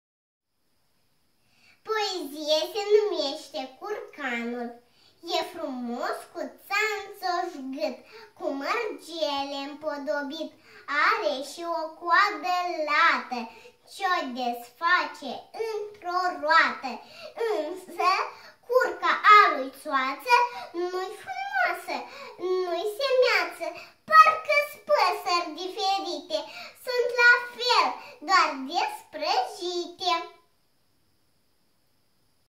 Anastasia Ursu - Curcanul_1 (fara autor)
Anastasia Ursu - Inimioara Noastra Mica!
Anastasia Ursu recita poezia - Curcanul_1 (fara autor).